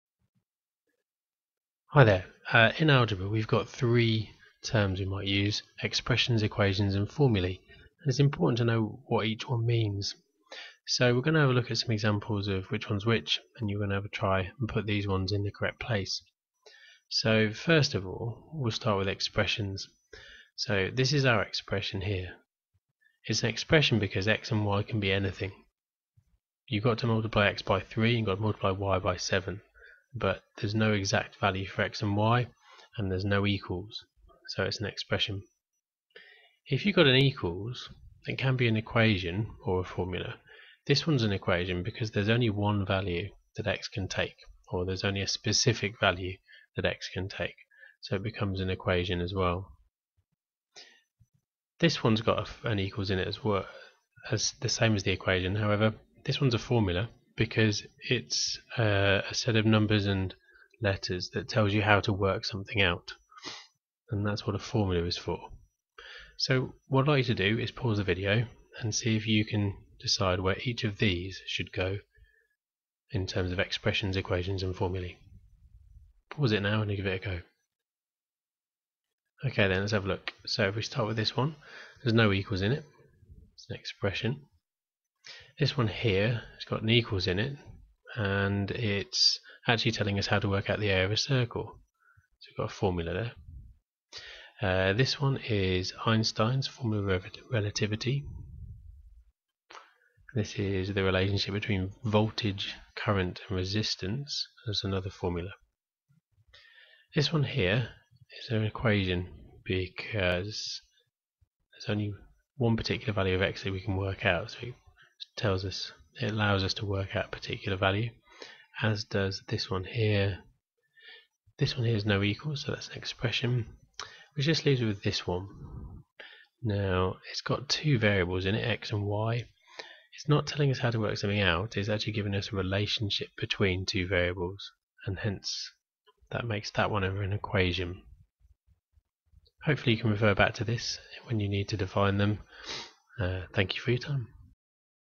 Expressions, equations and formulae
Quick explanation of the difference between expressions, equations and formulae